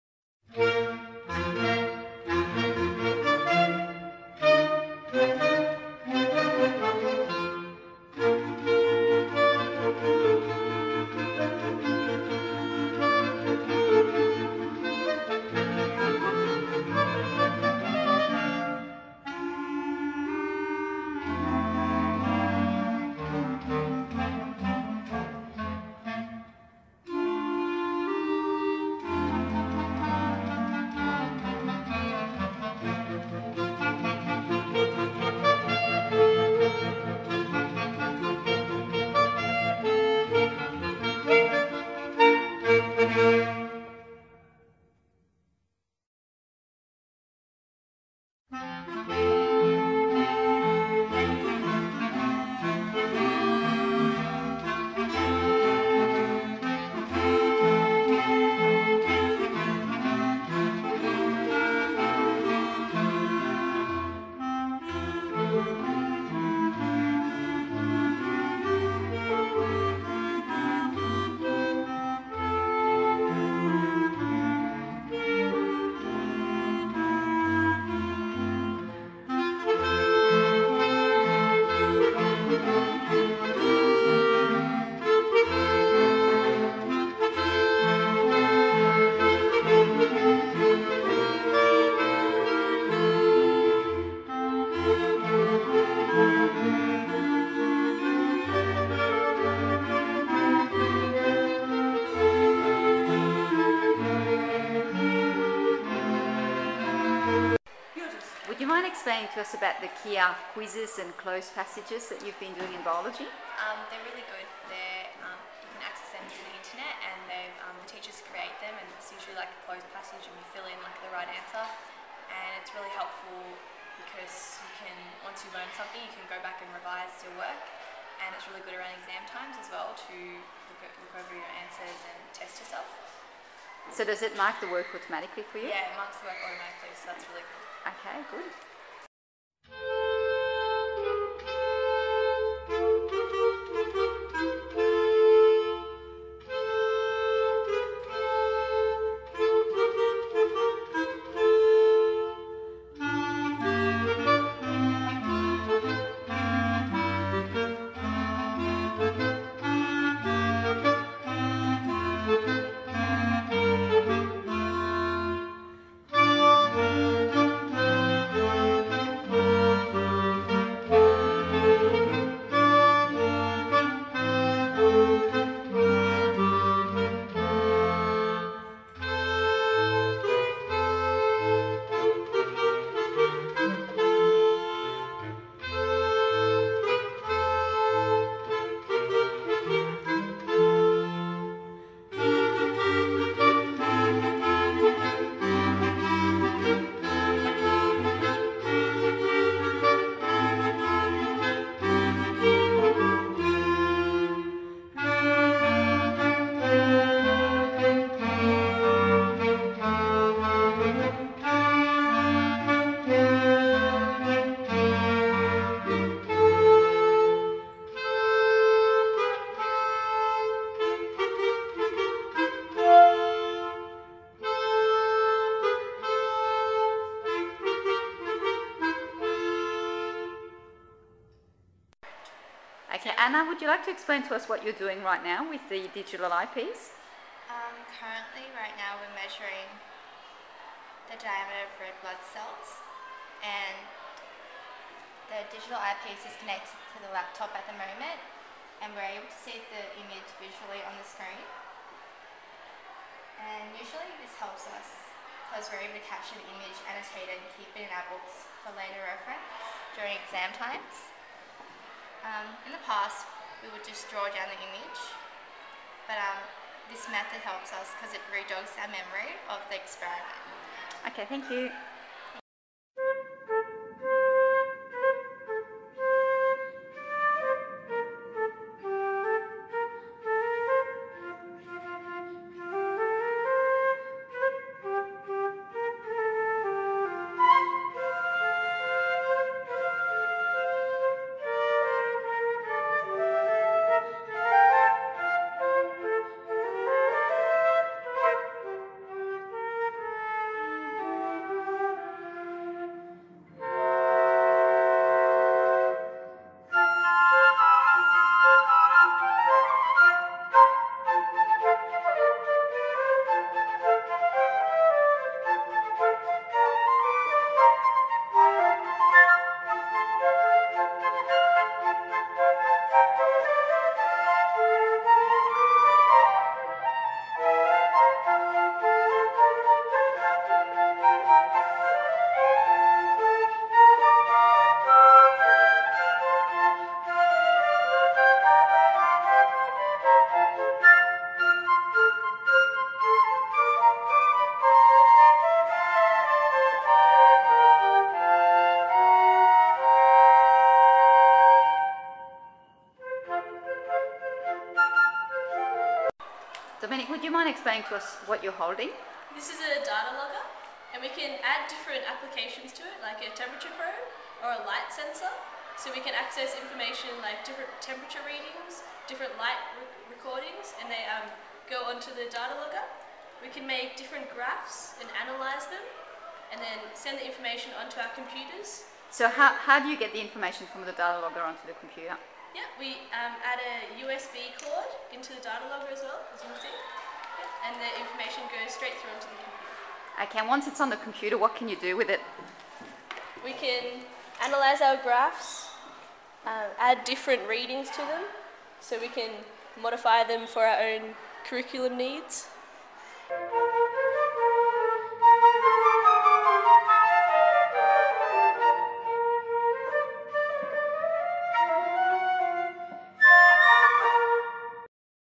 Redesigning the learning experience in Biology through the use of HP Tablet Computers
The Science faculty of Our Lady of Mercy College, Parramatta was very fortunate to be awarded a Hewlett Packard Innovation in Education Grant in 2009. At OLMC this project focused on redesigning the learning experience in the Biology classroom. Students have responded very positively to these. They allow students to include hand drawn diagrams as well as hand written chemical and mathematical equations to word processed lab reports and class notes. When used in conjunction with digital eye pieces, the tablets make it possible for students to easily annotate and draw over microscope images.